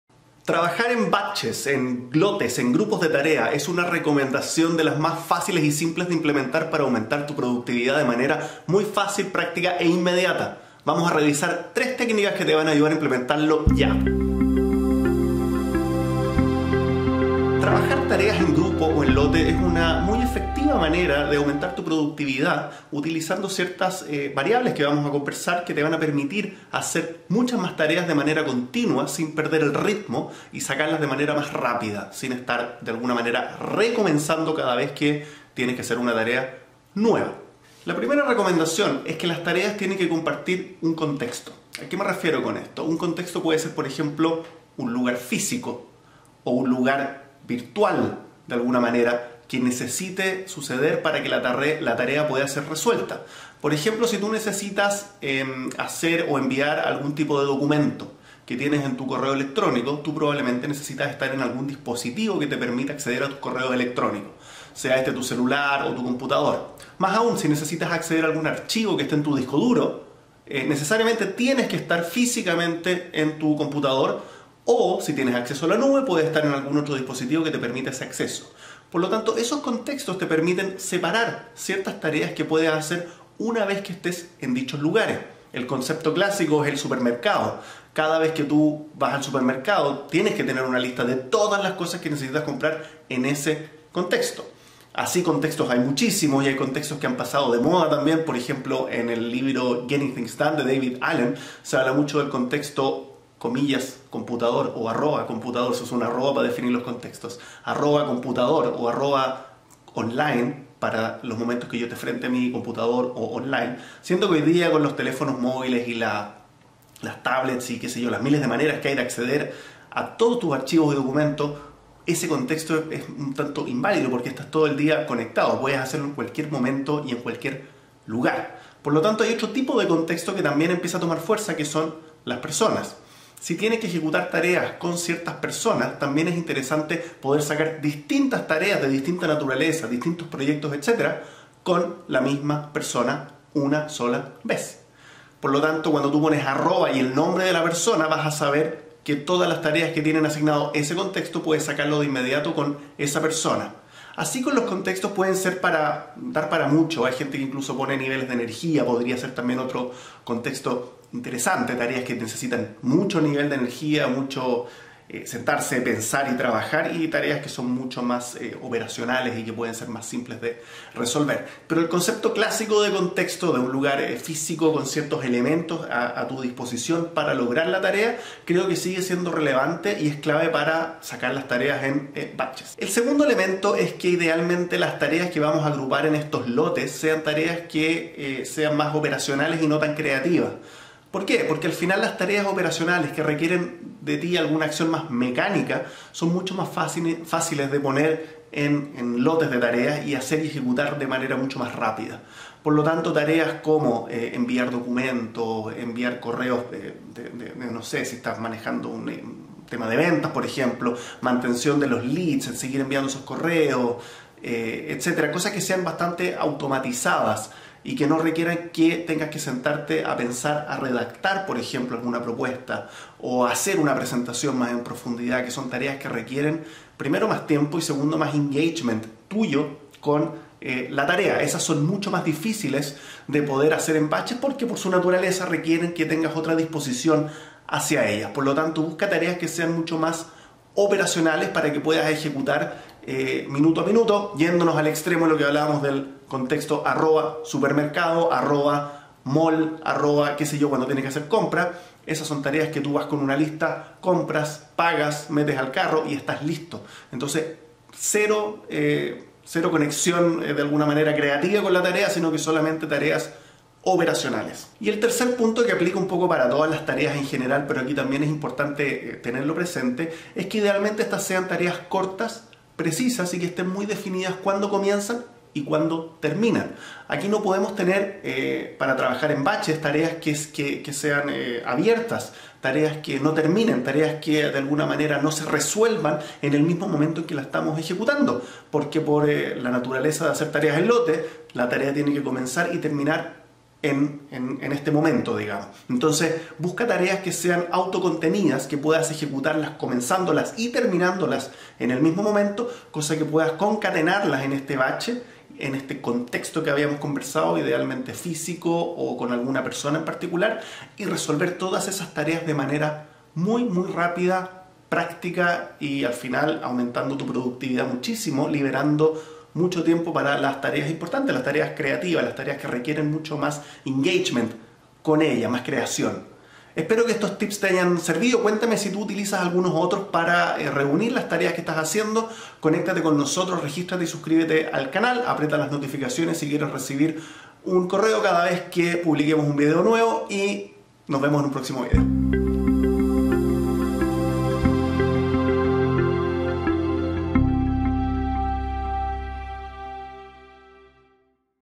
3 recomendaciones para trabajar tus tareas en ´batches´o lotes y ser más productivo
Tres claves para que el hacer tareas en lotes o "batches" te haga más productivo.